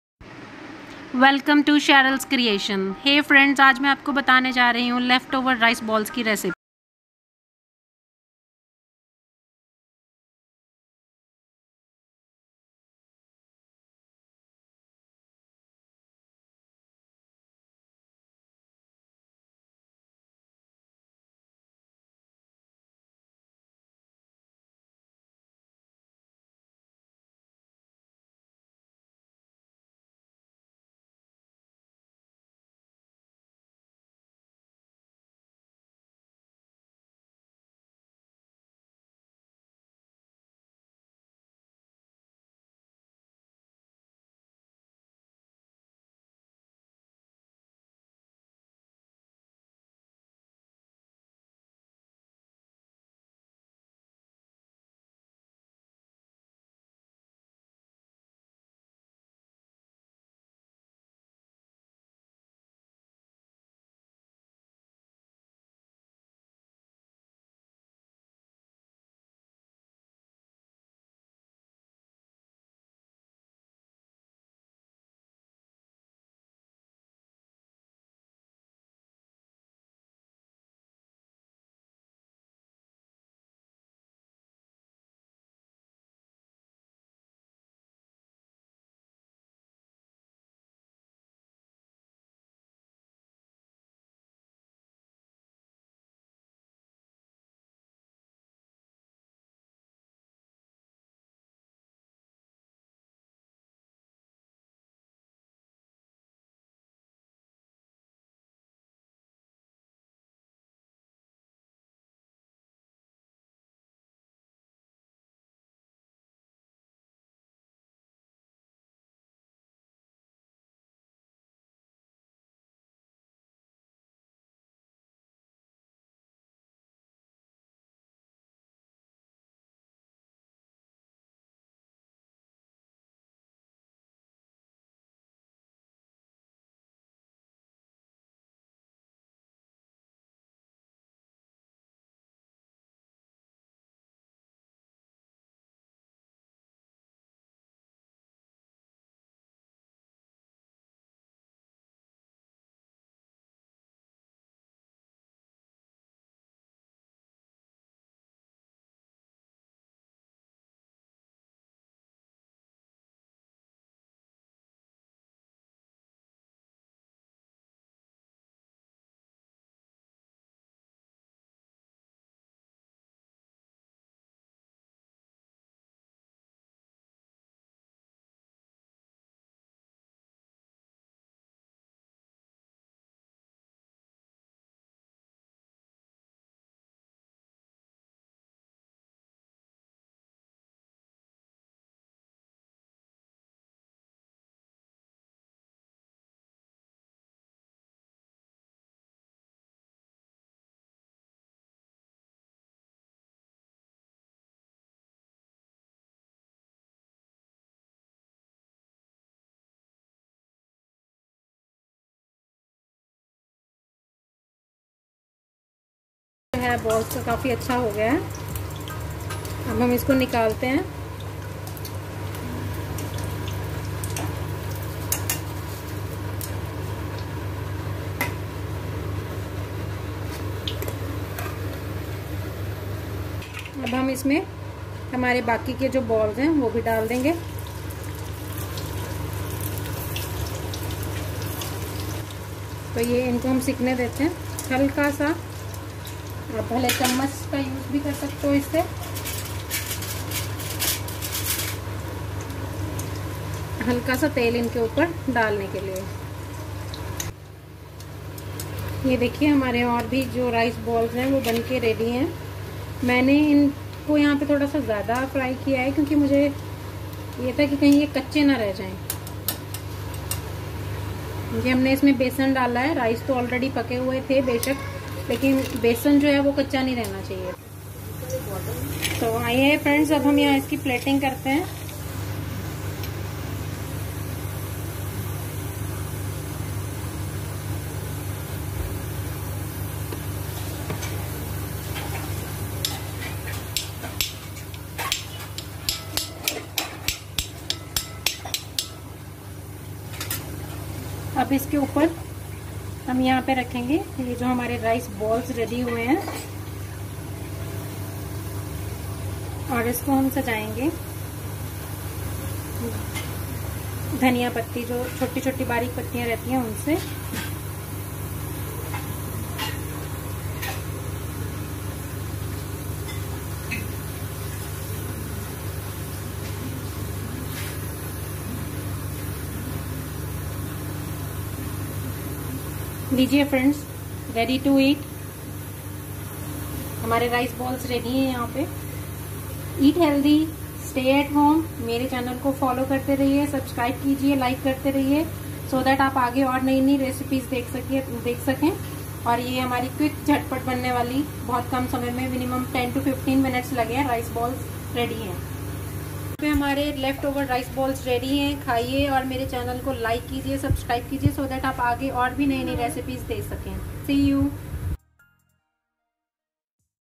🔥🌶Secret Recipe of Rice Ball, Dumplings, a Quick and tasty Snack.🔥🔥
An age old household recipe, on how to Make Rice Balls with Leftover rice.